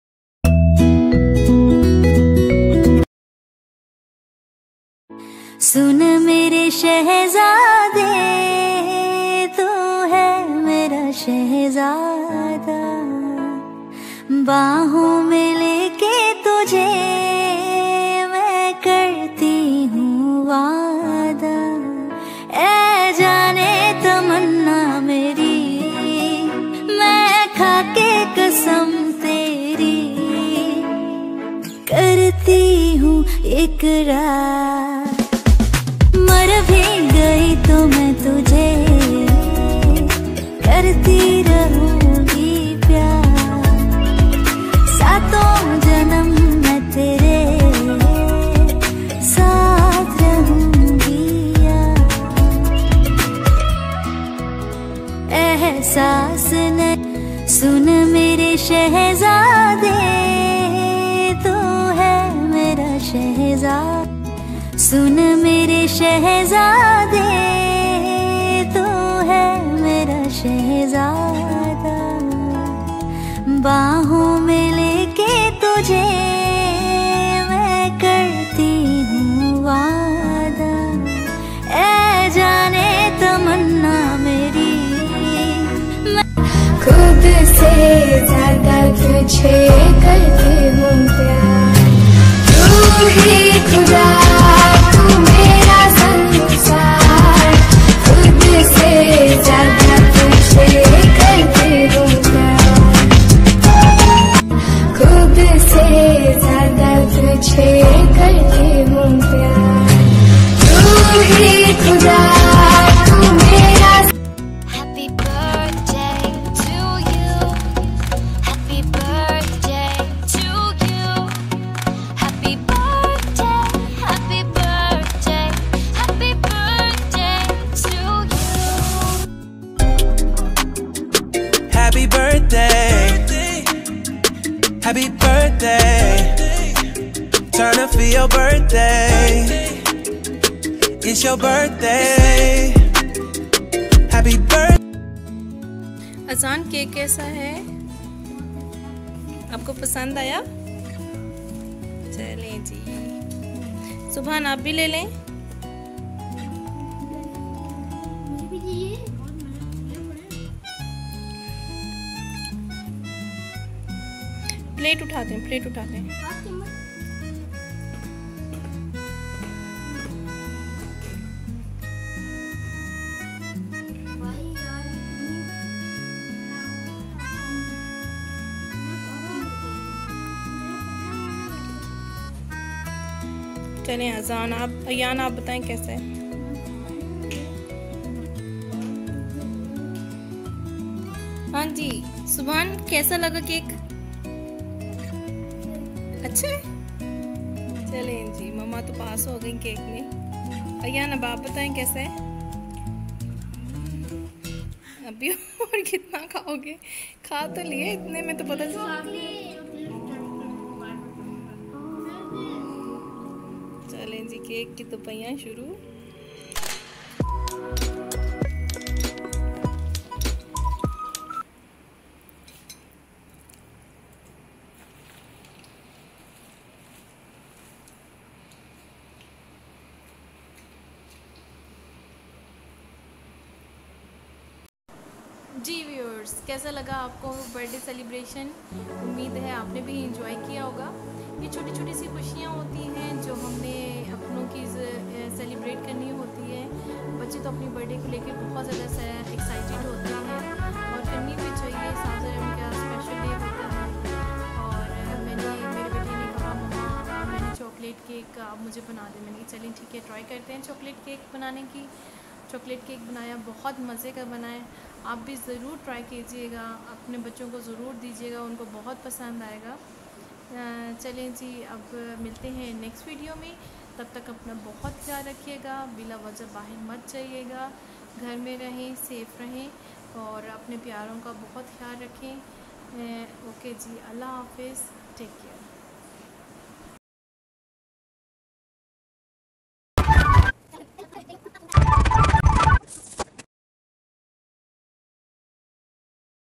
Azan's Birthday at home//ghar mn salgirah//mom_kiz// by Zobia Ansari
Azan's Birthday celebration at home. In first video I made cake for him.. Share some of his memorable pics...